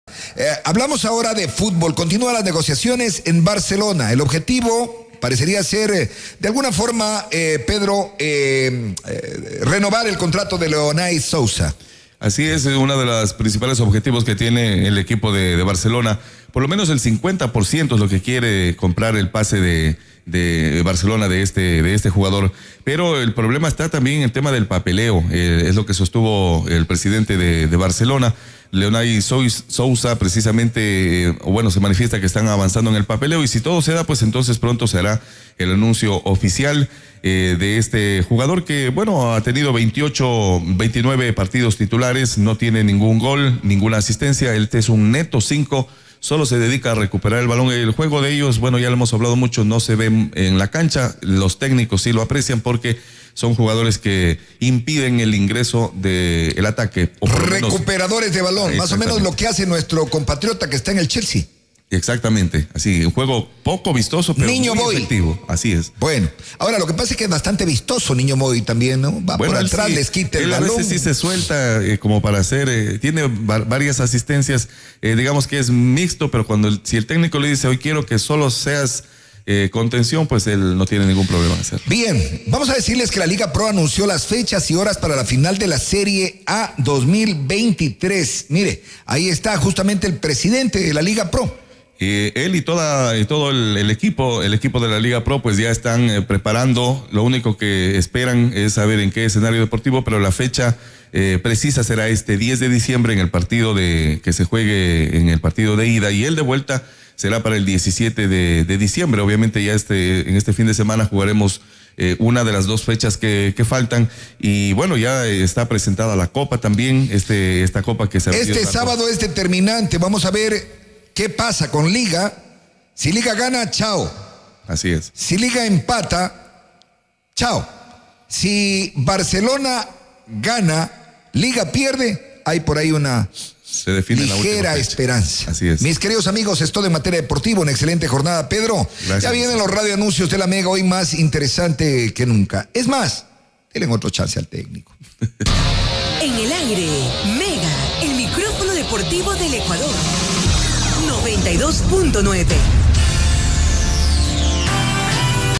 🚀 ¡NEGOCIACIONES! Barcelona SC busca RENOVAR el contrato de Leonai Souza 🔥 | ¿Liga en problemas?
Prepárate para un viaje al corazón de las negociaciones futbolísticas! Barcelona SC está en una misión para renovar el contrato de Leonai Souza, mientras Liga enfrenta posibles problemas. Descubre los detalles, estadísticas impactantes y la fecha clave para la final de la Serie A 2023. ¿Qué depara el destino para estos equipos? ¡No te lo pierdas! 🔥 ¡Dale clic ahora! ⚽

🔥 ¡Las negociaciones en Barcelona SC están en su punto álgido! Descubre los desafíos que enfrentan para renovar el contrato de Leonai Souza. Además, la Liga Pro anuncia fechas cruciales para la final de la Serie A 2023. ¿Quién se llevará la victoria? ¡Entérate de todo y sé testigo de la emoción del fútbol! ⚽ ¡Haz clic ya! 🔥